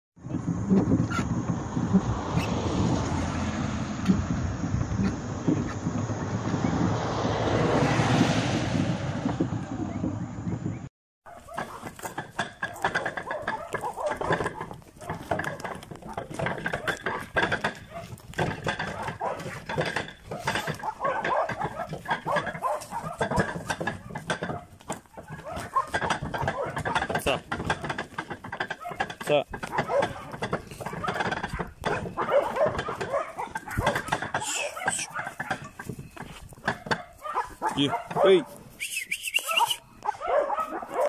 QUEMANDO FIEBRE NOCTURNA - Boyeotico CR 2023
Los boyeros aprovechando las frescas noches y salen a quemar fiebre con sus bueyes... cuidado el sonido los confunde con la leyenda de La Carreta Sin Bueyes...